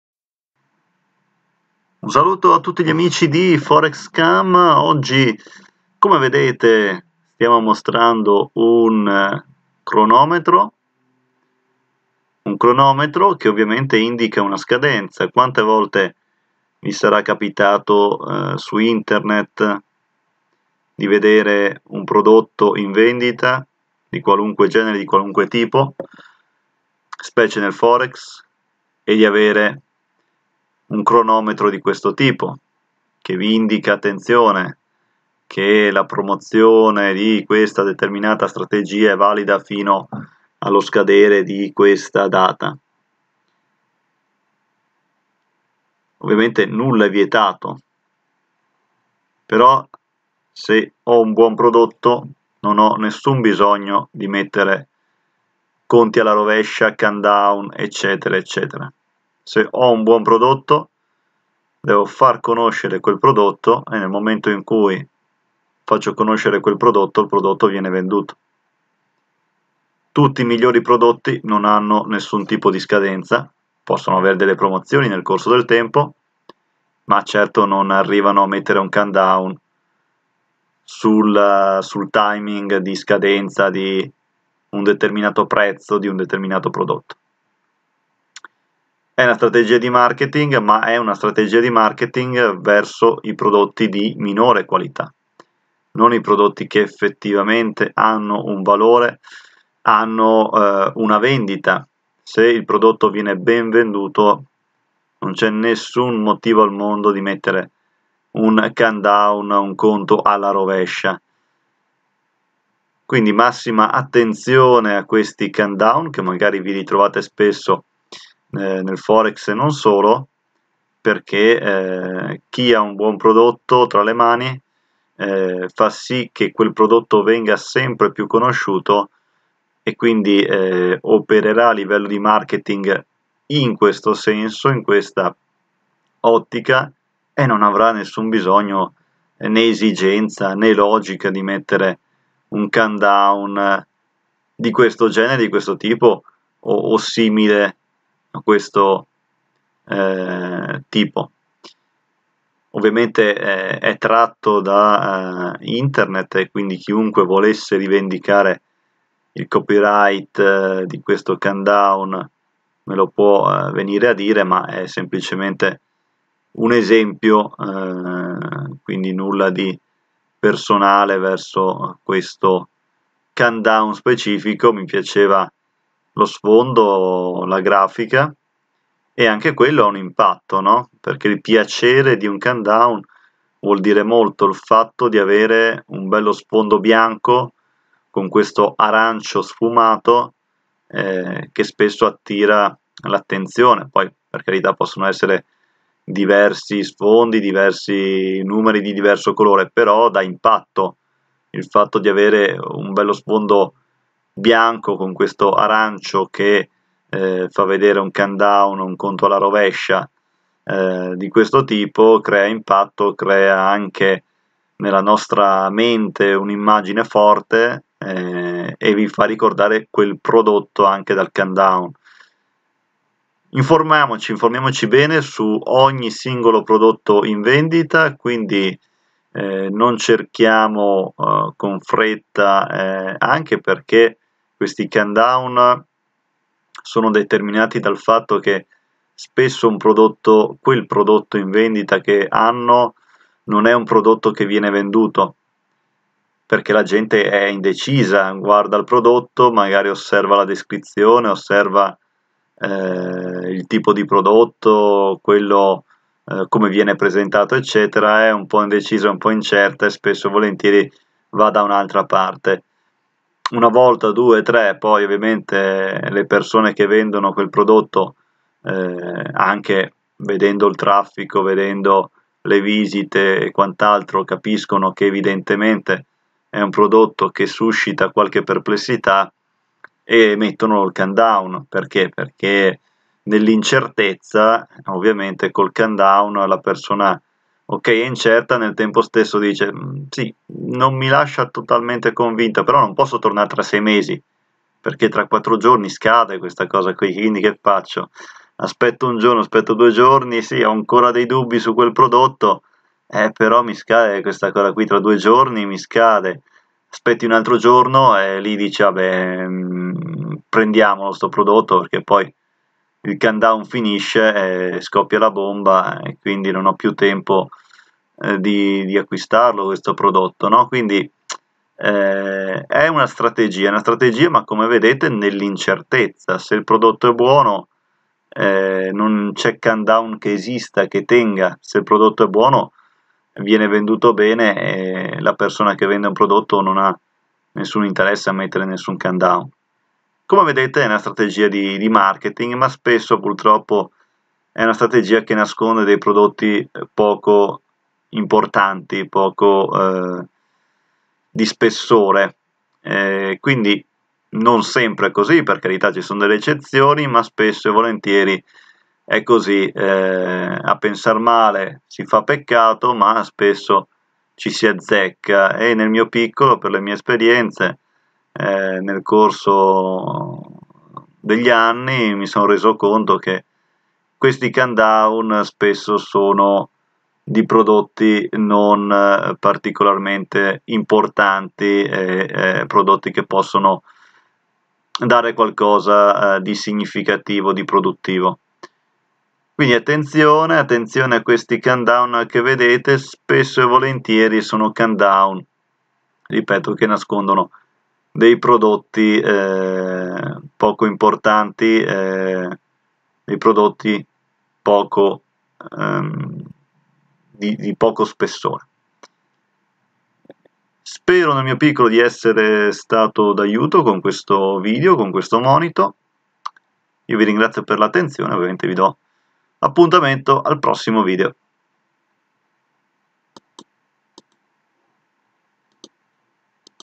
[FOREX SCAM] Nel trading gli unici conti che mostrano sono quelli alla rovescia
Quante volte vi sarà capitato di vedere questi conti alla rovescia? Nel trading online i conti attivi visibili sono quelli alla rovescia,quelli reali sono sempre piu rari purtroppo